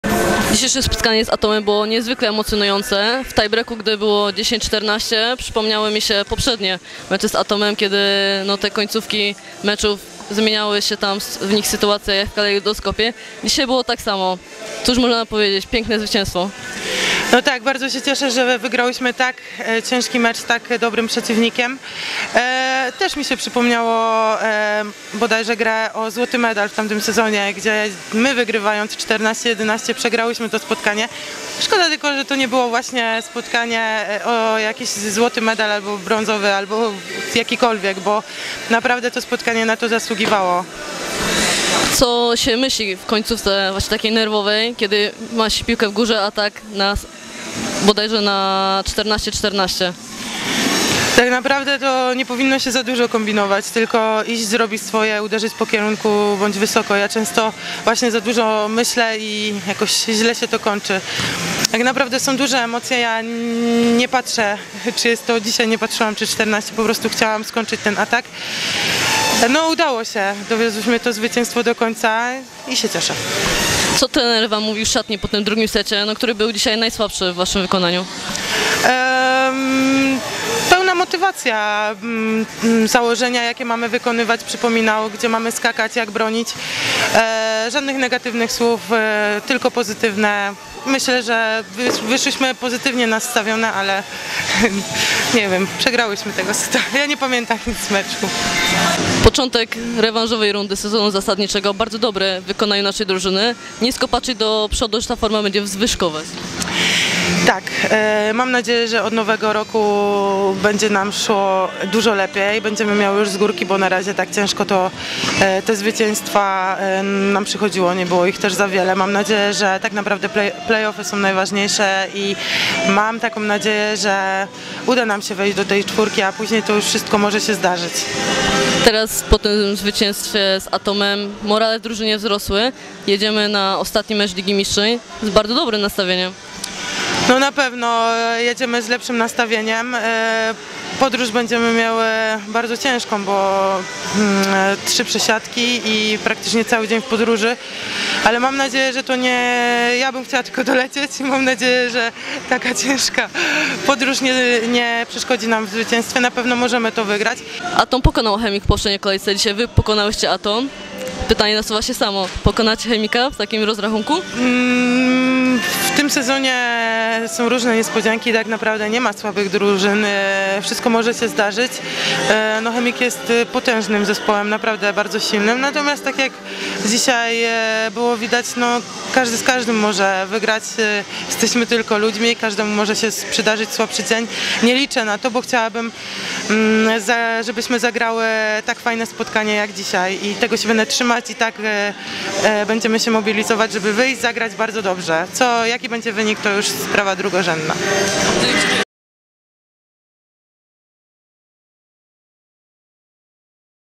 Wywiad z Katarzyną Zaroślińską
Wywiad z Katarzyną Zaroślińską po meczu TAURON MKS Dąbrowa Górnicza vs ATOM Trefl Sopot z dnia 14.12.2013